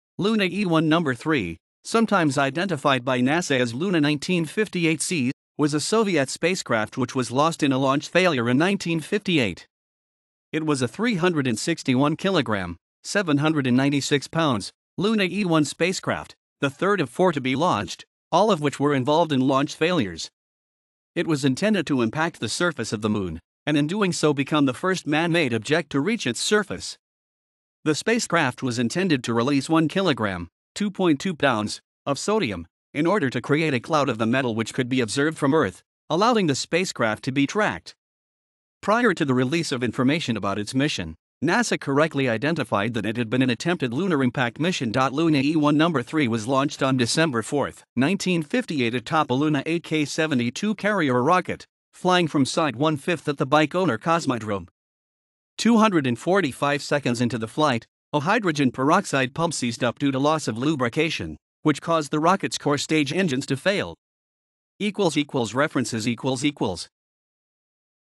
Luna E-1 No.3
Luna E-1 No.3, sometimes identified by NASA as Luna 1958C, was a Soviet spacecraft which was lost in a launch failure in 1958. It was a 361-kilogram (796 lb) Luna E-1 spacecraft, the third of four to be launched, all of which were involved in launch failures. It was intended to impact the surface of the Moon, and in doing so become the first man-made object to reach its surface.
The spacecraft was intended to release 1 kilogram (2.2 lb) of sodium, in order to create a cloud of the metal which could be observed from Earth, allowing the spacecraft to be tracked. Prior to the release of information about its mission, NASA correctly identified that it had been an attempted Lunar impact mission.Luna E-1 No.3 was launched on 4 December 1958 atop a Luna 8K72 carrier rocket, flying from Site 1/5 at the Baikonur Cosmodrome. Two hundred and forty five seconds into the flight, a hydrogen peroxide pump seized up due to loss of lubrication, which caused the rocket\s